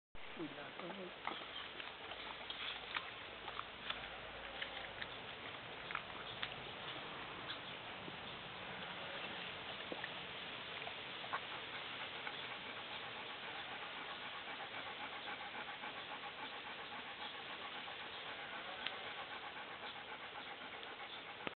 Maya got a figs
Maya got a nice figs.
She loves fruits :)